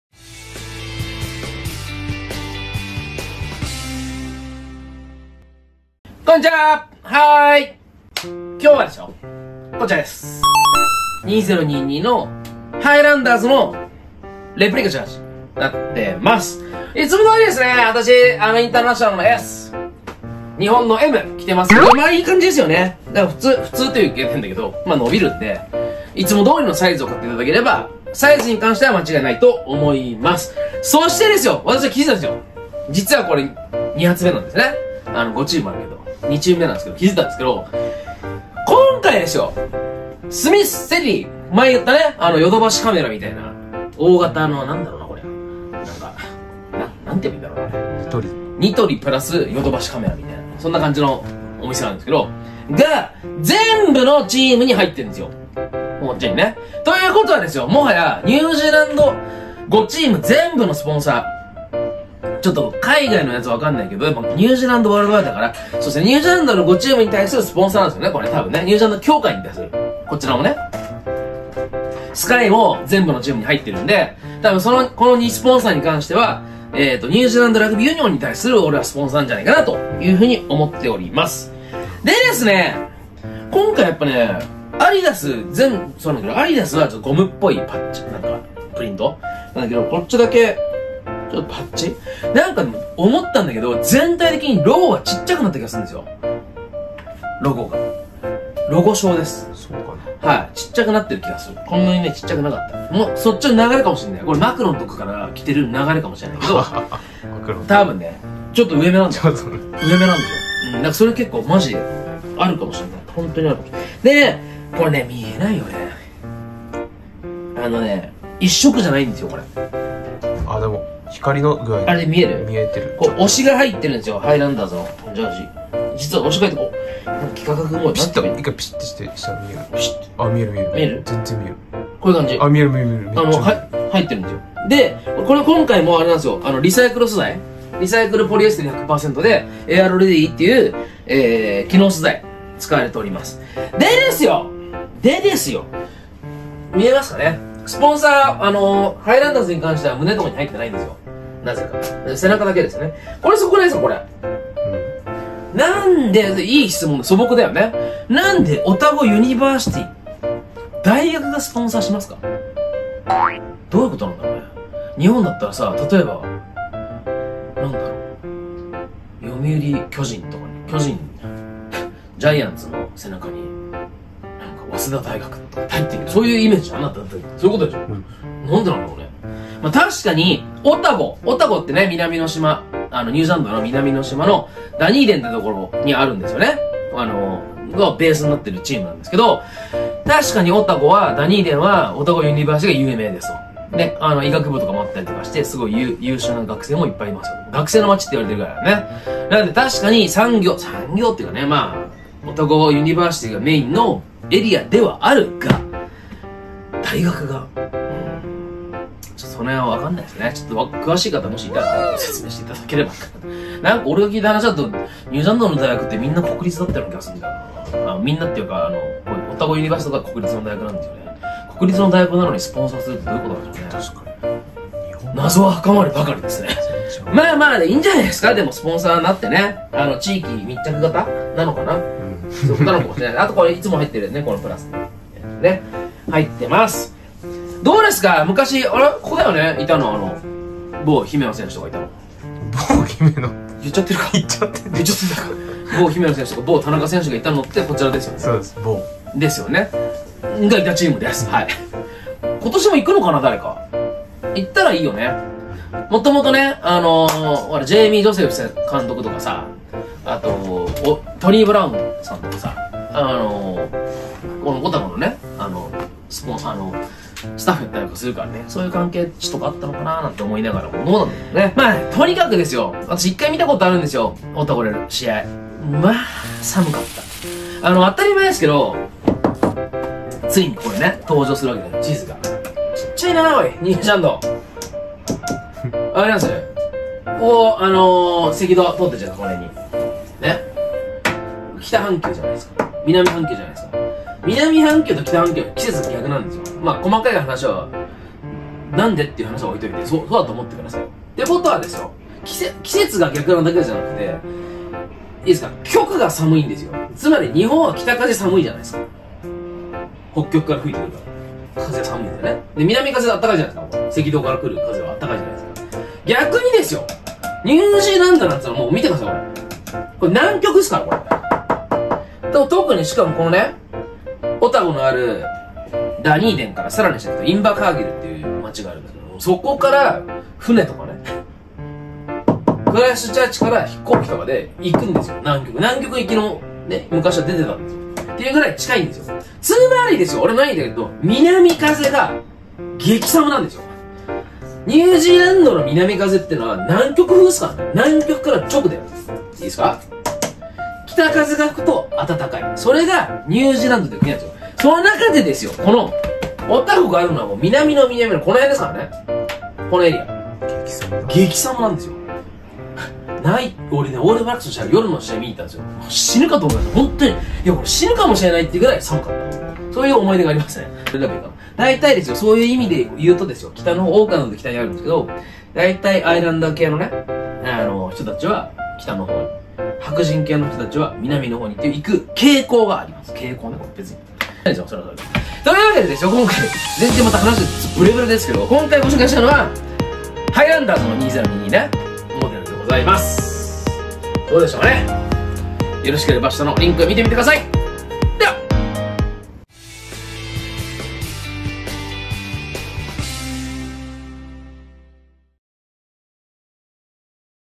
ハイランダーズ 22/23 ホームジャージ
アディダス製 SUPER RUGBY ハイランダーズ22/23年モデルのホームジャージです。

ラグビーオンラインでの販売ページはこちら！